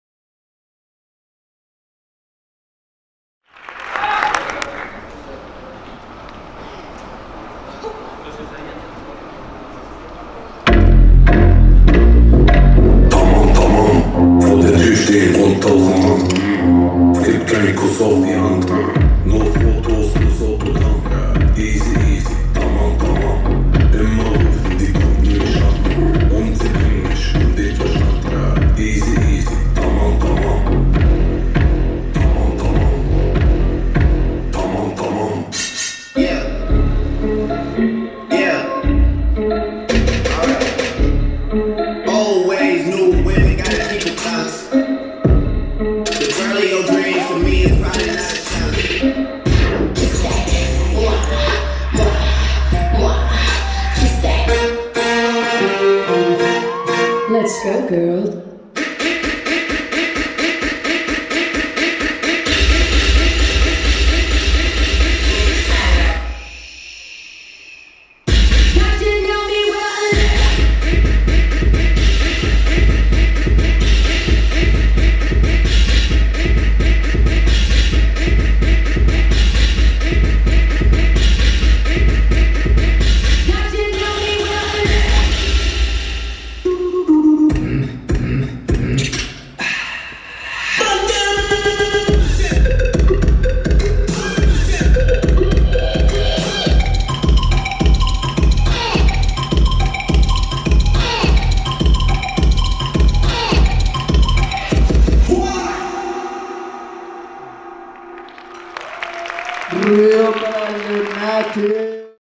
Lunatics - Edo. Mex. (Categoría MiniCrew) @ HHIMexico2019 Preliminares
Mexico Hip Hop Dance Championship 2019

Más de 170 grupos reunidos en la Ciudad de México compitiendo para poder representar a nuestro país en el Campeonato Mundial HHI 2019 en Phoenix, Arizona.

Fotografía y video: @theanswerfilms